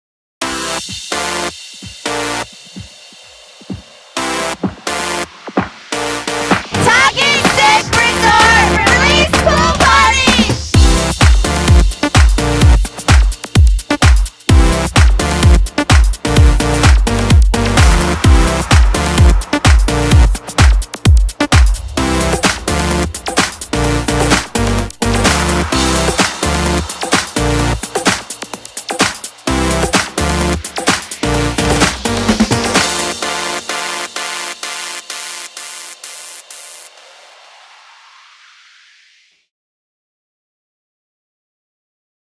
Release Summer Pool Party in Scottsdale | Talking Stick Resort
Beautiful people. Great drinks. VIP Cabanas. Live DJs.

Release summer pool parties are the perfect way to enjoy the sun, music, and great people at Talking Stick Resort's main pool. DJ's from Las Vegas, San Diego, San Francisco, and Los Angeles keep the energy going. You can party in style with drink specials, promotional giveaways, and cabana rentals.

The DJ line-up for the 2012 Release Pool Party:
DJ Viceroy
DJ Crooked
White Noise
DJ Echo
DJ Serafin
DJ Skratchy
DJ Este
DJ Gusto
DJ G Squared
DJ Turbulence
DJ ERock
DJ Cheapshot
Slick D
DJ Rick Rude

You'll have a hard time finding a more exciting pool party in Phoenix or Scottsdale. Get ready for the valley's best pool party at Talking Stick Resort's Release summer pool party, with better drinks, better VIP cabana options, and great music!

If you're looking forward to Release 2013, please "like" and share this great pool party video with your friends.